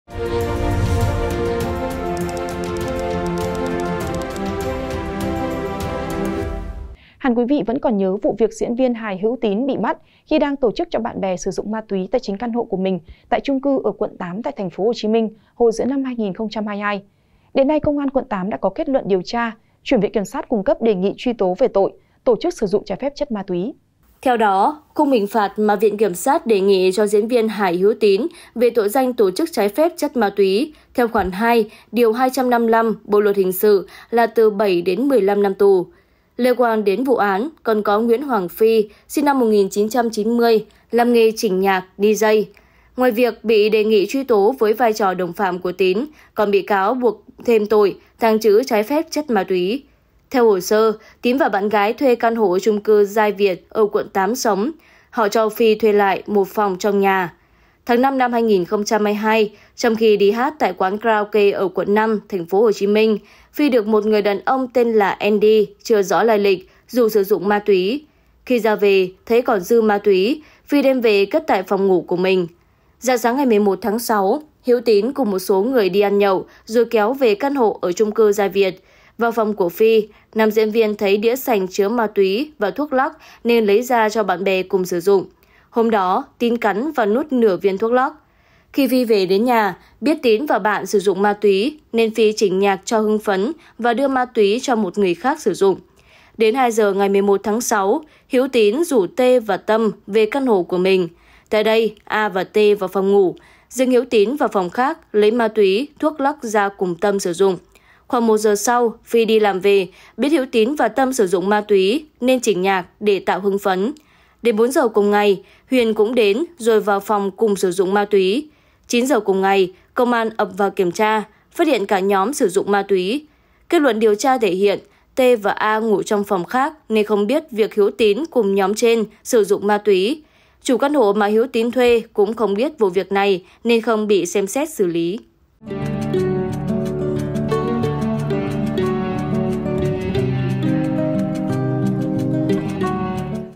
VKS Đề Nghị Truy Tố Diễn Viên Hữu Tín 7-15 Năm Tù | SKĐS
SKĐS | Cơ quan cảnh sát điều tra Công an quận 8 (TPHCM), đã hoàn tất kết luận điều tra, đề nghị truy tố bị can Trần Hữu Tín (35 tuổi, tức diễn viên Hữu Tín) về tội Tổ chức sử dụng trái phép chất ma túy  theo khoản 2 điều 255 bộ luật hình sự là từ 7- 15 năm tù.

Mời quý vị và các bạn xem thêm: Thảm Kịch Itaewon 100.00 Người Mắc Kẹt, 156 Người Tử Vong : Những Con Số Thống Kê Đầy Ám Ảnh